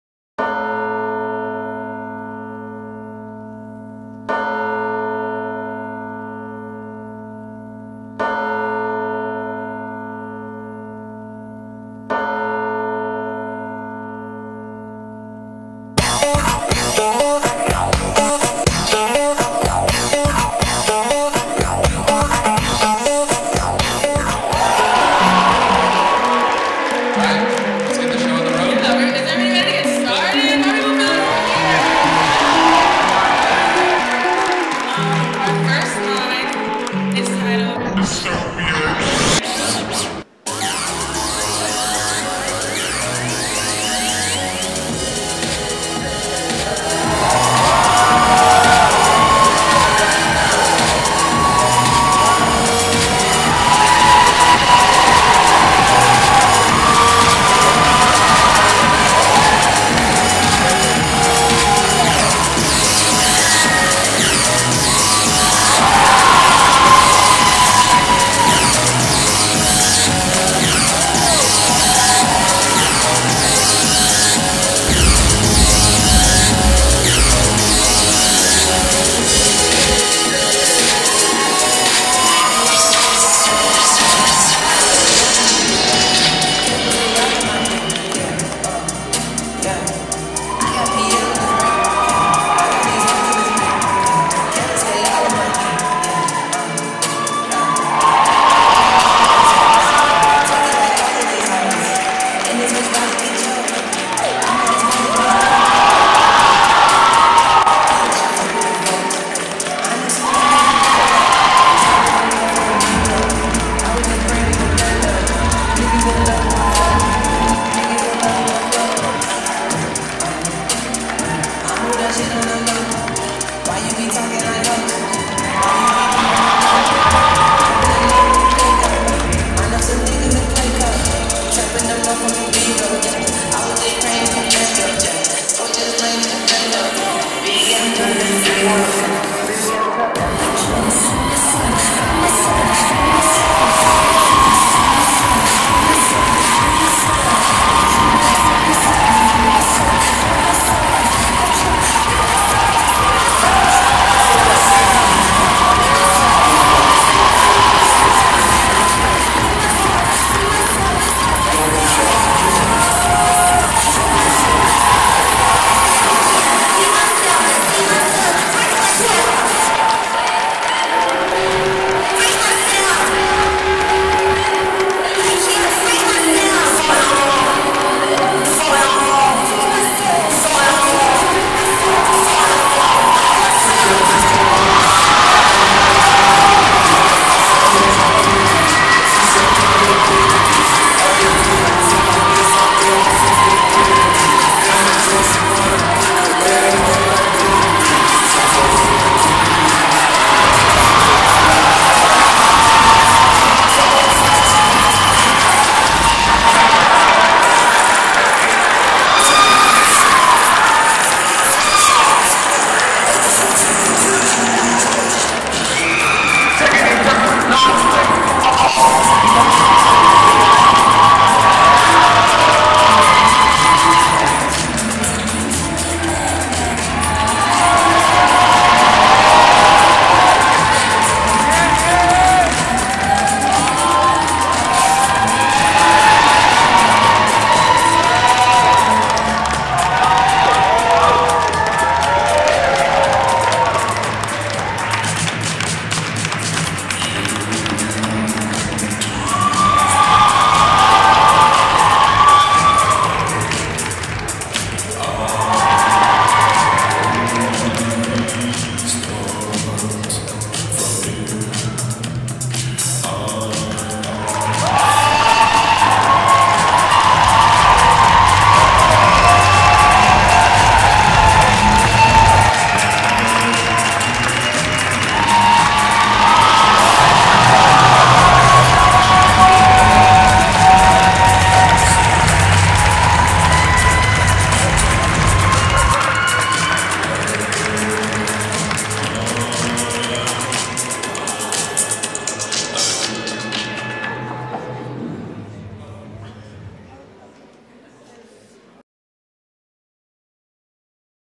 "DYSTOPIAN" - SOC Fashion Show 2024
"DYSTOPIAN"
A fashion line created by Erick Buendia
Showcased at the SOC Fashion Show 2024 at Wesleyan